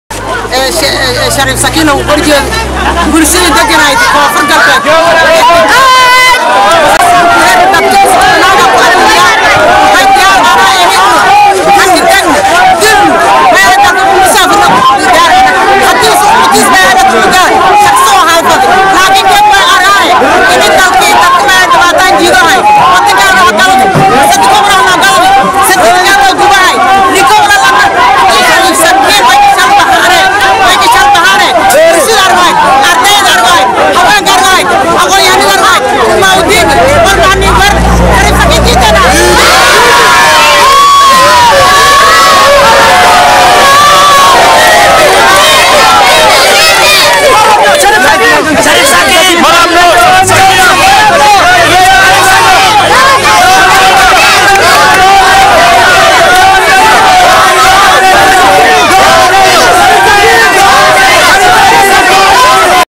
BAYDHABO 2014 - MURAAYO WACDARO KA MUUJISEY MUDAAHARAADKII BAYDHABO - DAAWO OO DHAGEEYSO
BAIDOANEWS  AMAANDHOOREY OO WAXBA IDIINKA QARIN DOONIN DHACDOOYINKII KA DHACAY BAYDHABO 2014 LA SOCDA MAR WALBA IYO MEEL WALBA